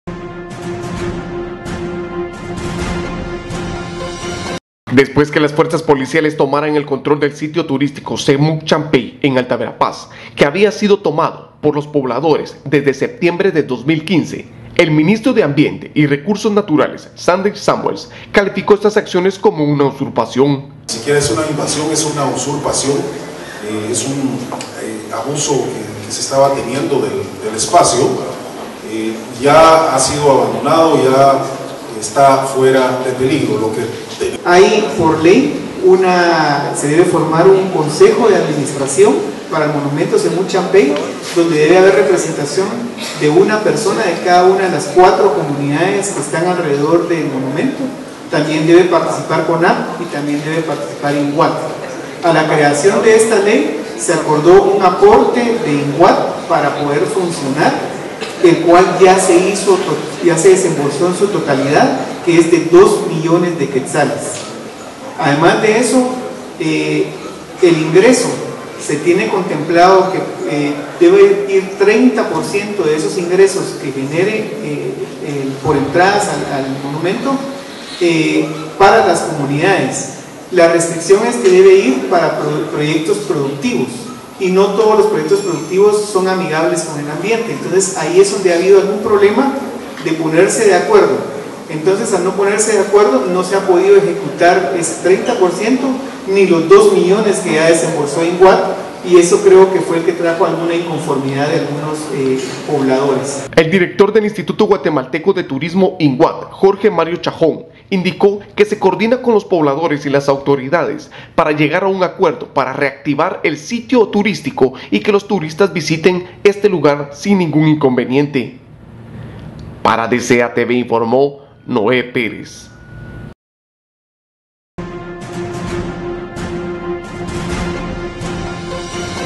ESPERAN CONSENSO PARA REACTIVAR EL SITIO TURISTICO SEMUC CHAMPEY
El director del Instituto Guatemalteco de Turismo INGUAT, Jorge Mario Cajón, índico que se coordina con los pobladores y las autoridades para llegar a un acuerdo para reactivar el Sitio turístico y que los Turistas visiten este lugar sin ningún inconveniente